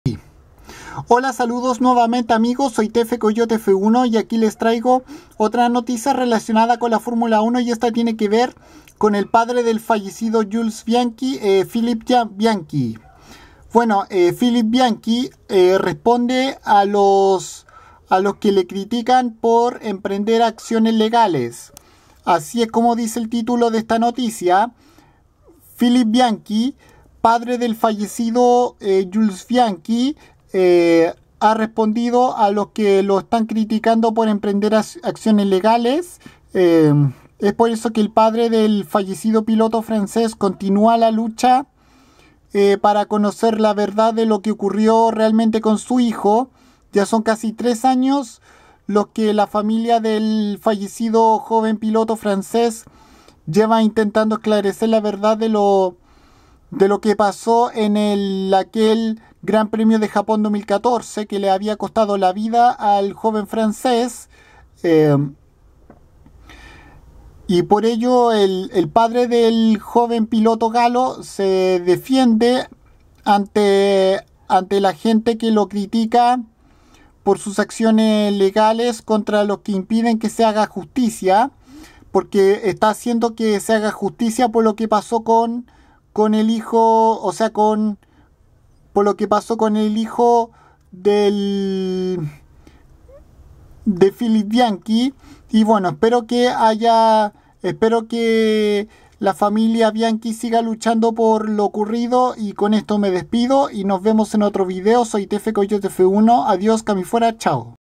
Phillipe Bianchi ha respondido a quienes lo critican por emprender acciones legales
El padre del fallecido piloto francés Jules Bianchi,Phillipe,le ha respondido a la gente que lo critica por emprender acciones legales y continua por saber la verdad por lo ocurrido con su hijo en el fatal Gp de Japón de 2014 que lo dejó en coma 9 meses hasta su muerte